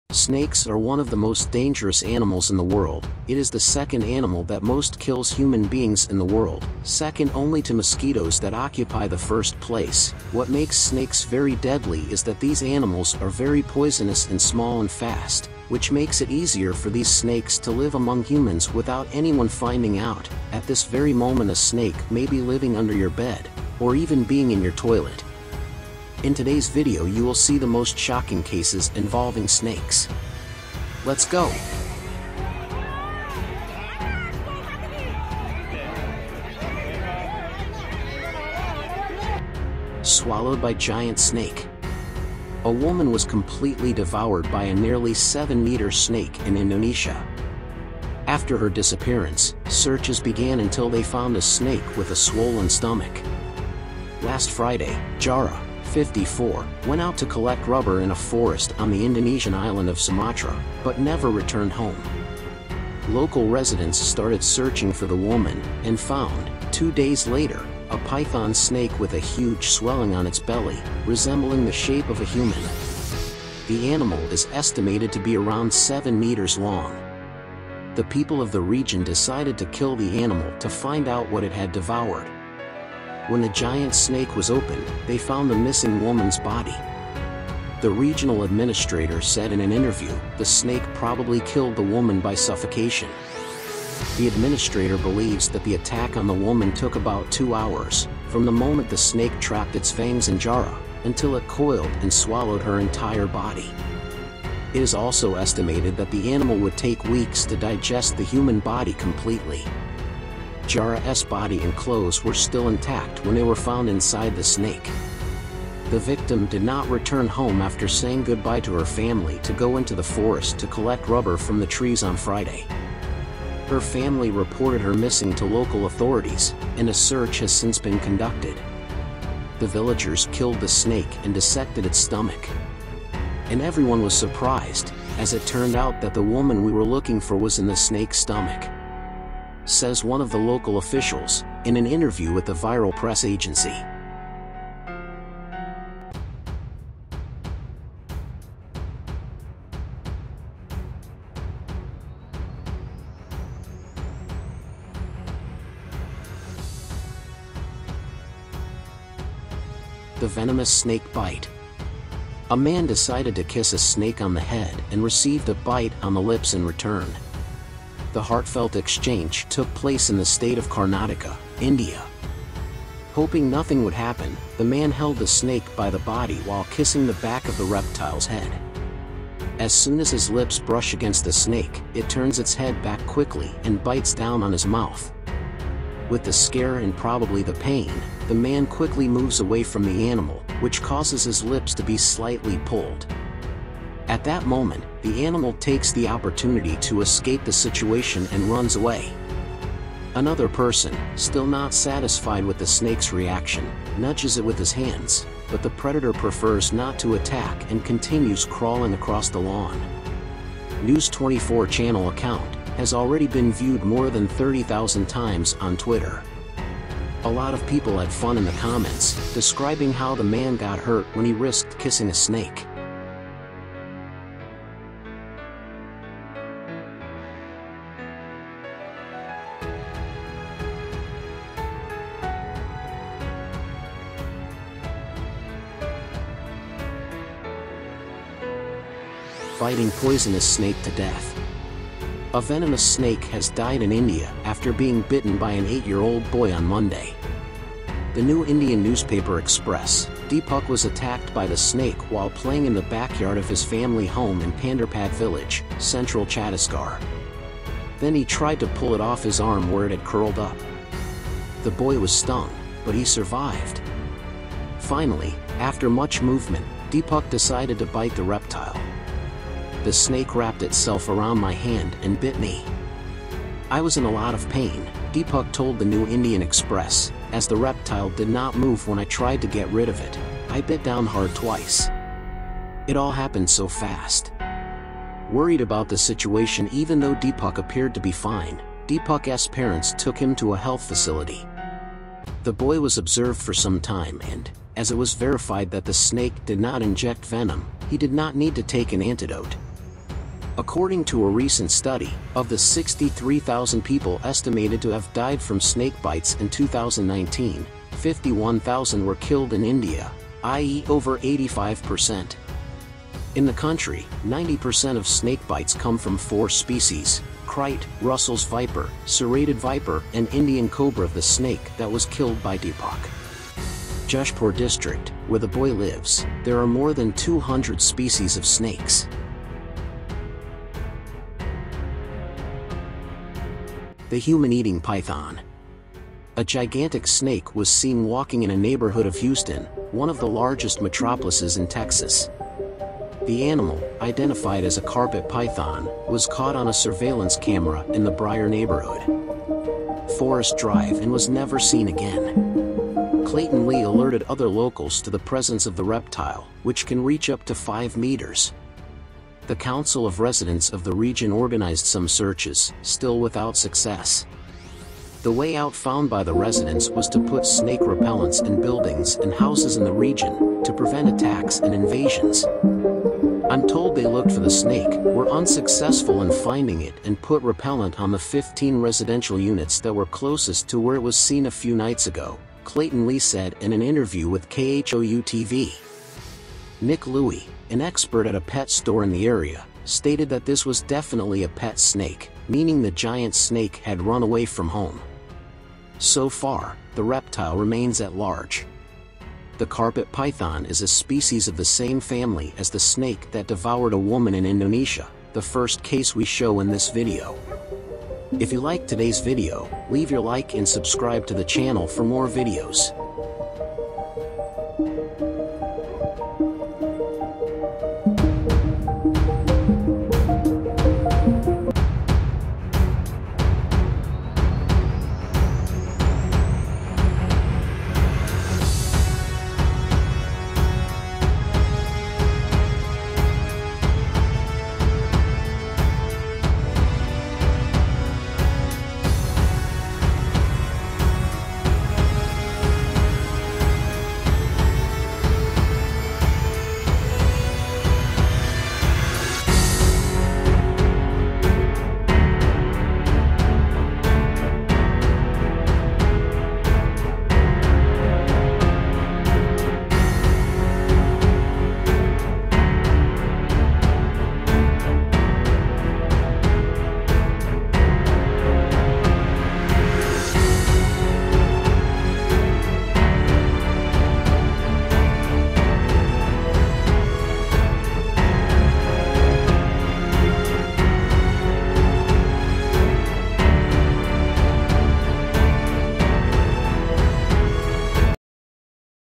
SHE WAS SWALLOWED ALIVE BY A GIANT PYTON AND THE WORST HAPPENED
Snakes are one of the most dangerous animals in the world, it is the second animal that most kills human beings in the world, second only to mosquitoes that occupy the first place, what makes snakes very deadly is that these animals are very poisonous and small and fast, which makes it easier for these snakes to live among humans without anyone finding out, at this very moment a snake may be living under your bed, or even being in your toilet. In today's video you will see the most shocking cases involving snakes. Let's go.

Swallowed by giant snake.
A woman was completely devoured by a nearly 7-meter snake in Indonesia. After her disappearance , searches began until they found a snake with a swollen stomach.
Last Friday, Jahrah , 54, went out to collect rubber in a forest on the Indonesian island of Sumatra, but never returned home.
Local residents started searching for the woman, and found, two days later, a python snake with a huge swelling on its belly, resembling the shape of a human. The animal is estimated to be around 7 meters long.
The people of the region decided to kill the animal to find out what it had devoured. When the giant snake was opened, they found the missing woman's body. the regional administrator said in an interview, the snake probably killed the woman by suffocation.
The administrator believes that the attack on the woman took about two hours, from the moment the snake trapped its fangs in Jahrah , until it coiled and swallowed her entire body.
It is also estimated that the animal would take weeks to digest the human body completely. Jahrah 's body and clothes were still intact when they were found inside the snake.
The victim did not return home after saying goodbye to her family to go into the forest to collect rubber from the trees on Friday. Her family reported her missing to local authorities, and a search has since been conducted. The villagers killed the snake and dissected its stomach. And everyone was surprised, as it turned out that the woman we were looking for was in the snake's stomach. says one of the local officials, in an interview with the ViralPress agency .

The venomous snake bite .
A man decided to kiss a snake on the head and received a bite on the lips in return. The heartfelt exchange took place in the state of Karnataka , India.
Hoping nothing would happen, the man held the snake by the body while kissing the back of the reptile's head. As soon as his lips brush against the snake, it turns its head back quickly and bites down on his mouth.
With the scare and probably the pain, the man quickly moves away from the animal, which causes his lips to be slightly pulled. At that moment, the animal takes the opportunity to escape the situation and runs away.
Another person, still not satisfied with the snake's reaction , nudges it with his hands, but the predator prefers not to attack and continues crawling across the lawn.
News 24 channel account , has already been viewed more than 30,000 times on Twitter . A lot of people had fun in the comments, describing how the man got hurt when he risked kissing a snake.